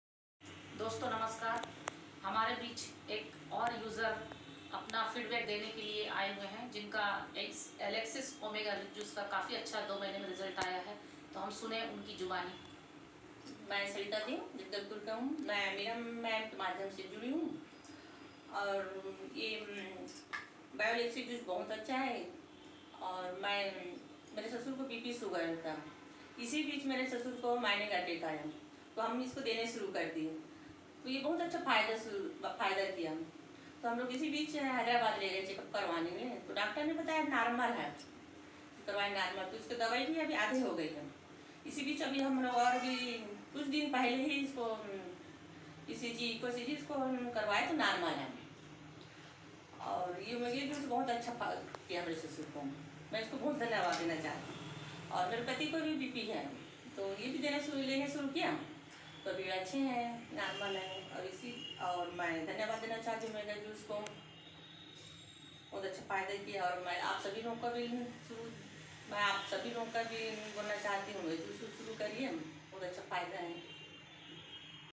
बी. पी. शुगर और अटैक की समस्या में अलेक्सिस ओमेगा रिच जूस के सेवन से बहुत फायदे मिले .
अलेक्सिस जूस प्रोडक्ट एक लेकिन इसके फायदे अनेक हैं .